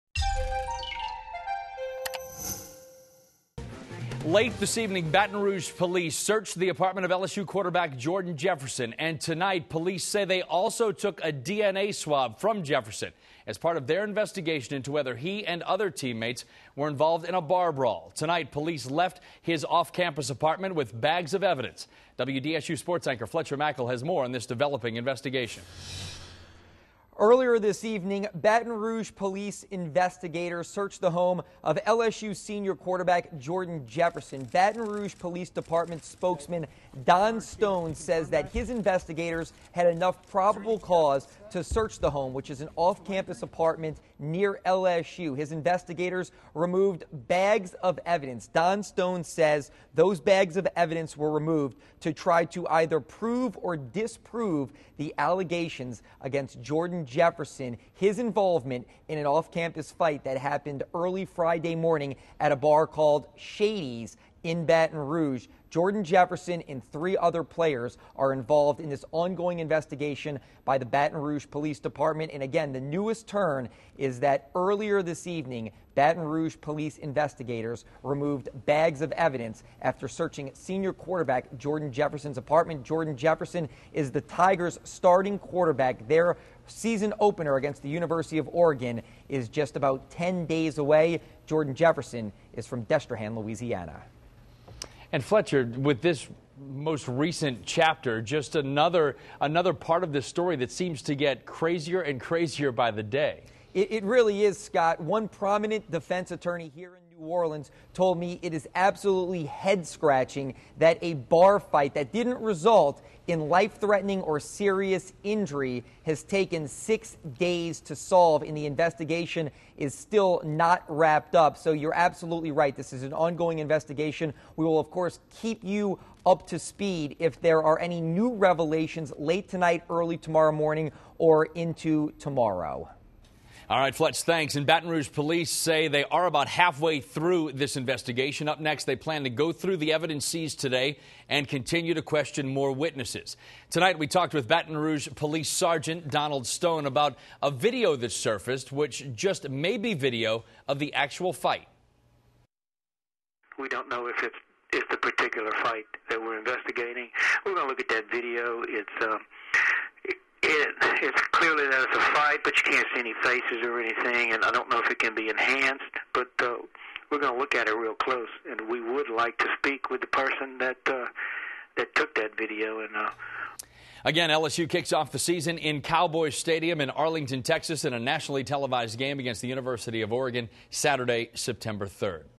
Police Search Home Of Star LSU Quarterback
Baton Rouge Police searched Louisiana State University quarterback Jordan Jefferson's home for evidence pertaining to the ongoing investigation of his involvement in a bar fight.